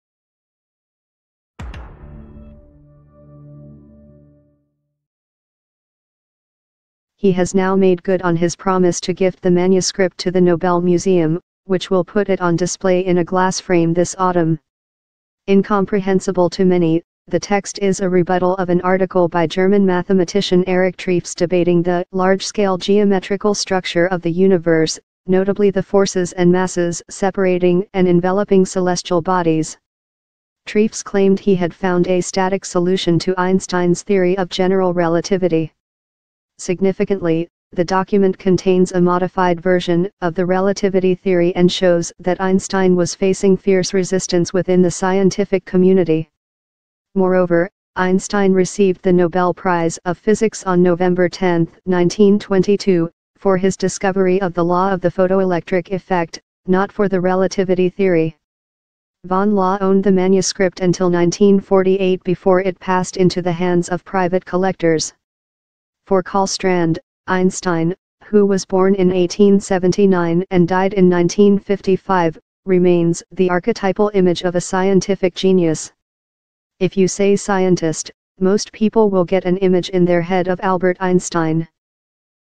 Web Page Paper At An Auction House For 1.2 Millionth Kroon (110,000 Euros) In December Recent Year.
He has now done nice on his pledge to souvenir the script to the Nobel Museum, that will put it on screen in a window box the fall.